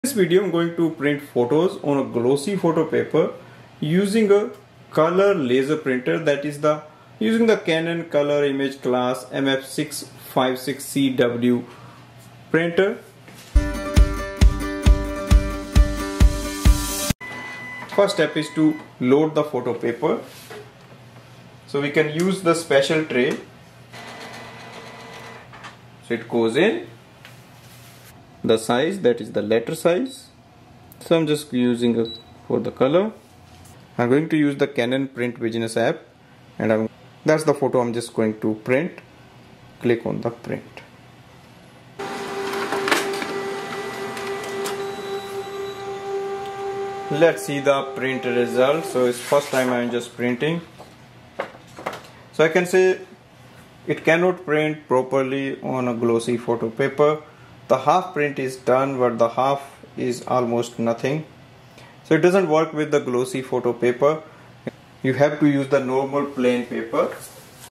Can You Print Photos On Glossy Paper With Canon Colour Laser Printer ?
This video reviews the print quality of the photos on glossy photo paper with your Canon colour Imageclass MF656Cdw colour All-in-one Laser printer. This shows how to print photos and whether should you use glossy photo paper or not ?.